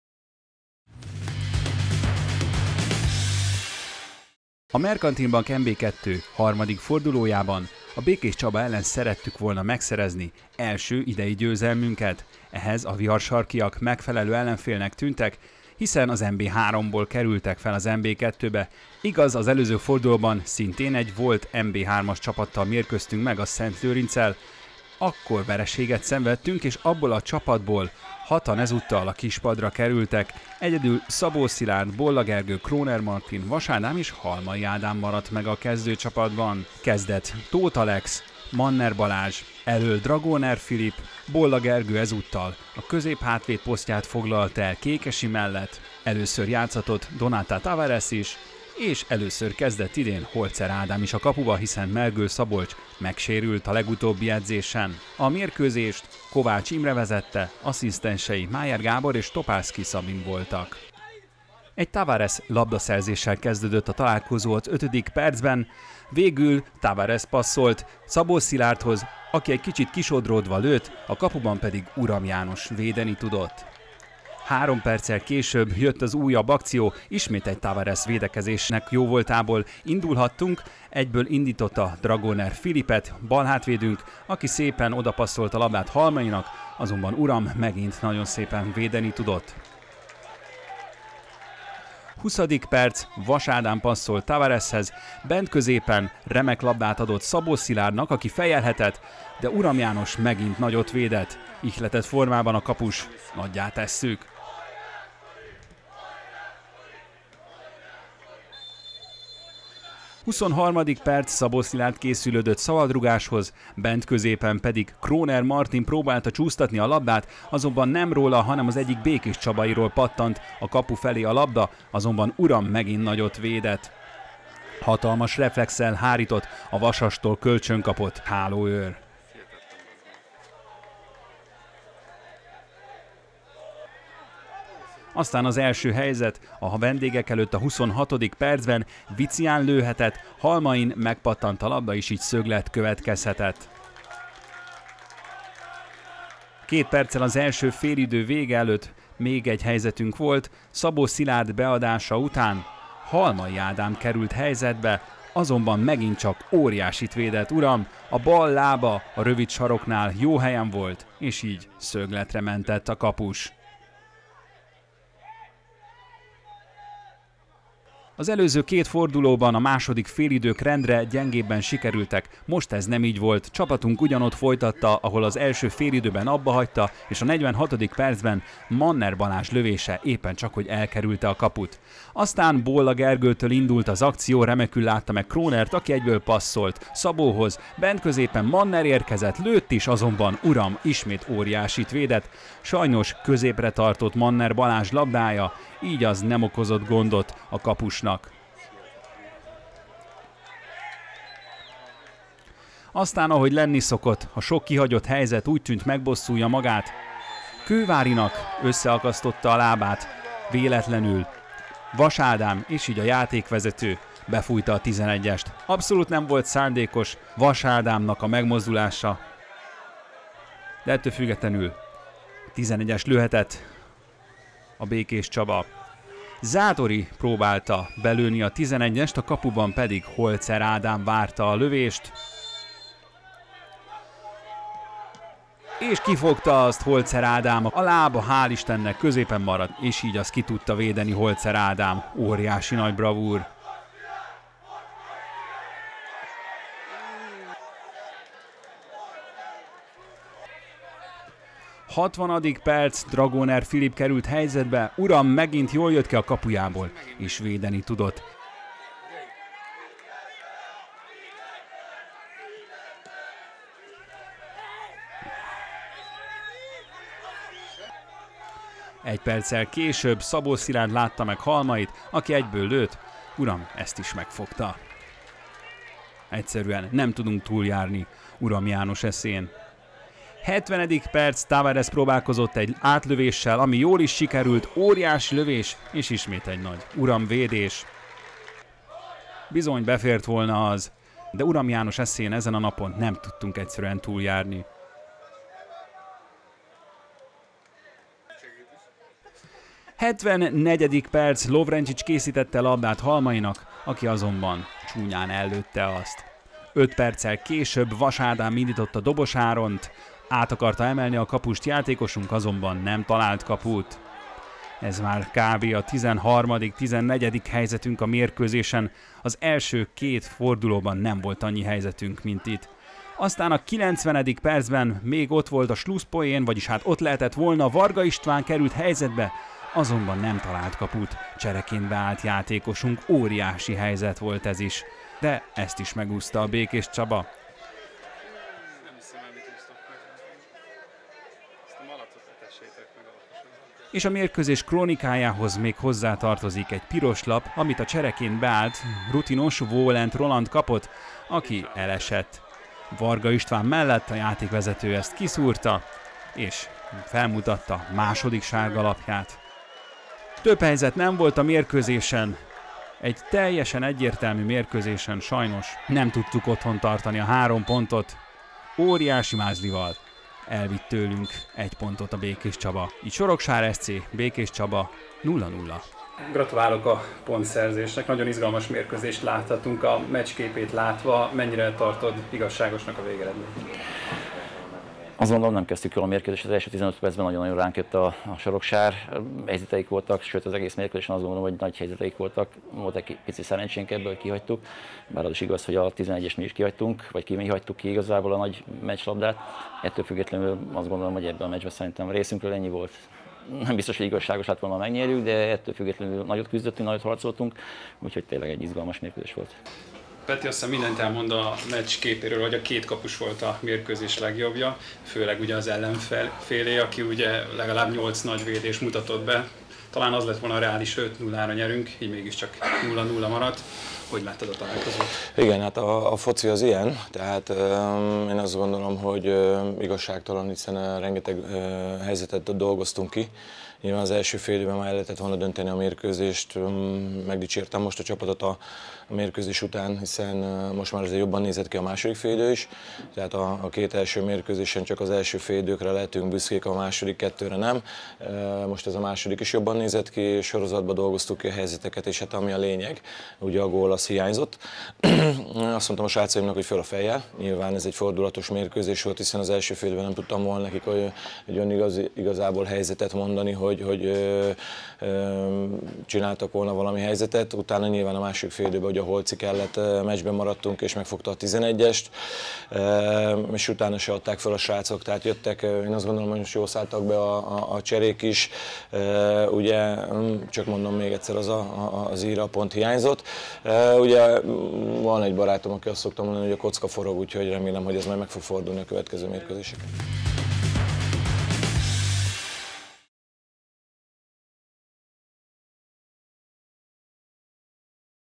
Soroksár SC - Békéscsaba 1912 előre | 0-0 (0-0) | Merkantil Bank Liga NB II. | 3. forduló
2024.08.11. Merkantil Bank Liga NB II. | 3. forduló | 2024/25 | MLSZTV
Soroksár SC - Békéscsaba 1912 előre | 0-0 (0-0) |
összefoglaló